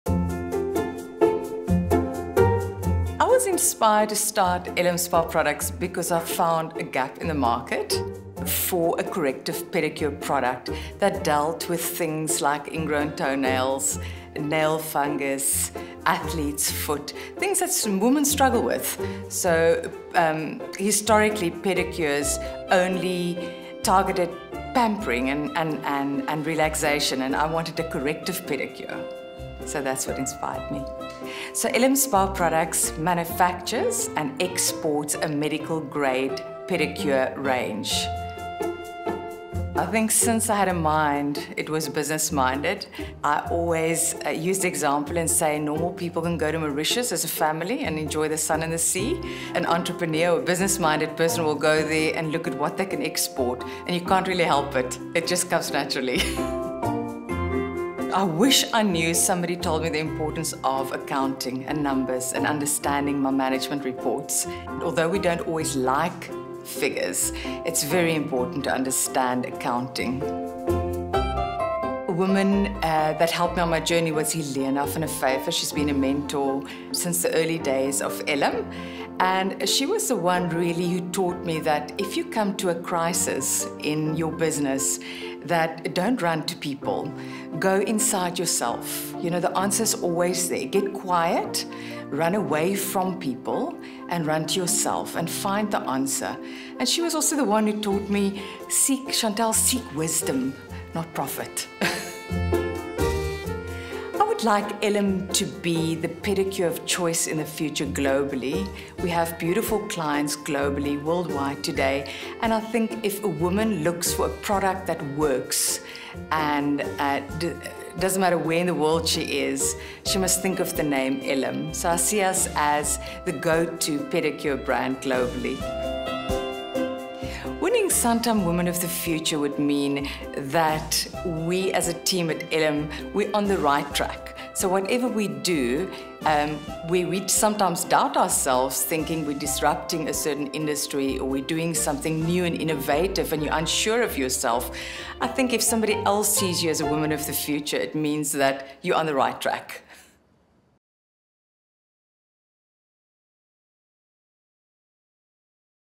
Santam Woman of the Future finalist 2024 l Shantelle Long-Booyens of Elim Spa Products
The 10th annual Santam Women of the Future Awards, in association with FAIRLADY and TRUELOVE, took place on Friday, 11 October, at an exclusive luncheon hosted at The Venue at the Houghton Hotel and emceed by Khutso Theledi.
Shantelle Long-Booyens of Elim Spa Products was a finalist in the Santam Woman of the Future category.